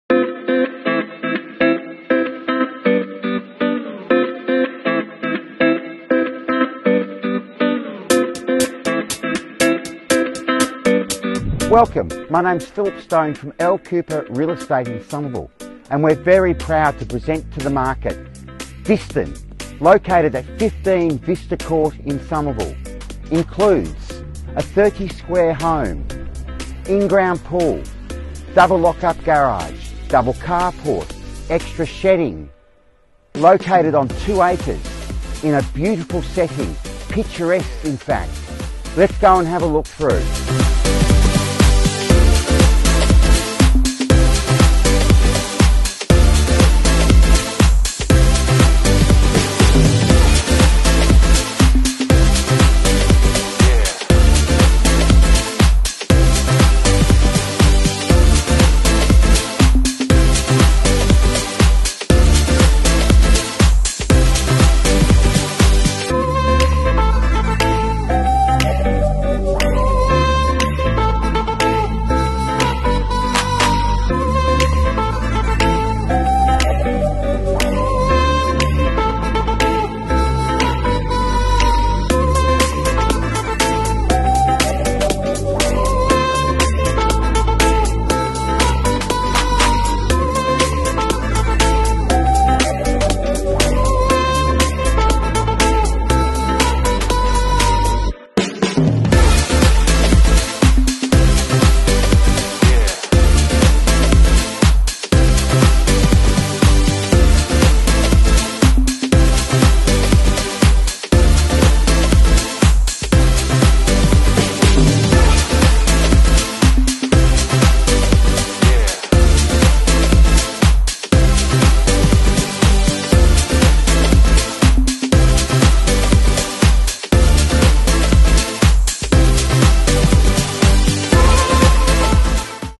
15 Vista Court, Somerville
Nestled privately  next to Vista Reserve, this well built builder’s own and loved family home is now offered to the market for the first time.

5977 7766